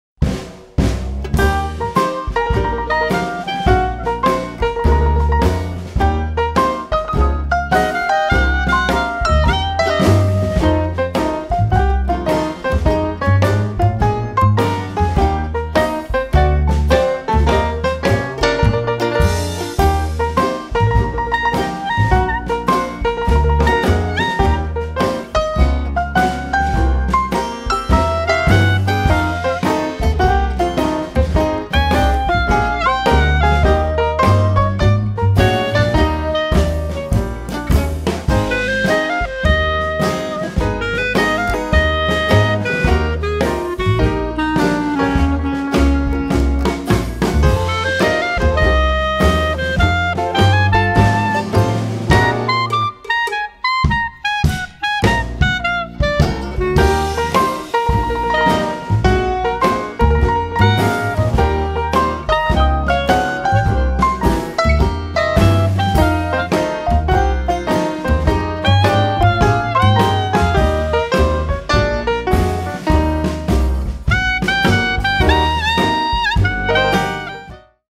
Come fare le ciliegie sciroppate - videoricette di frutta sciroppata
Allrecipes.it presenta la videoricetta delle "Ciliegie sciroppate". Leggi la ricetta su

Le ciliegie, si sa, una tira l'altra e in men che non si dica sono finite! Ma per godere di questo dolcissimo frutto tutto l'anno, conservatele sciroppate. Ottime per decorare dolci o insaporire gelati.

Autore: John Deley and the 41